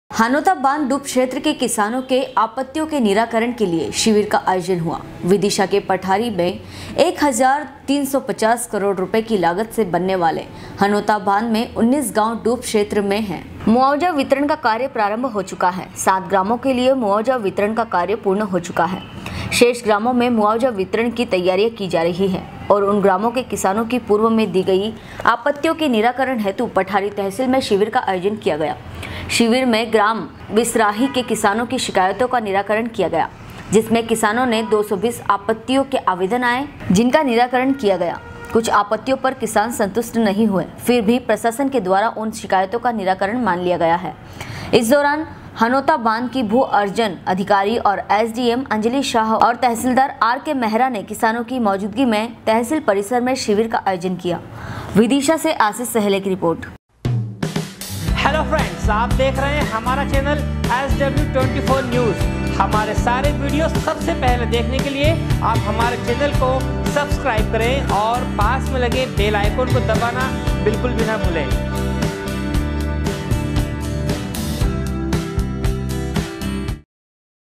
SW 24 News Madhyapradesh
हनोता बांध डूब क्षेत्र के किसानों के आपत्तियों के निराकरण के लिए शिविर का आयोजन हुआ । विदिशा के पठारी मे एक हजार 350 करोड़ रुपए की लागत से बनने वाले हनोता बांध में 19 गांव डूब क्षेत्र में है। विदिशा से आशीष सहेले की रिपोर्ट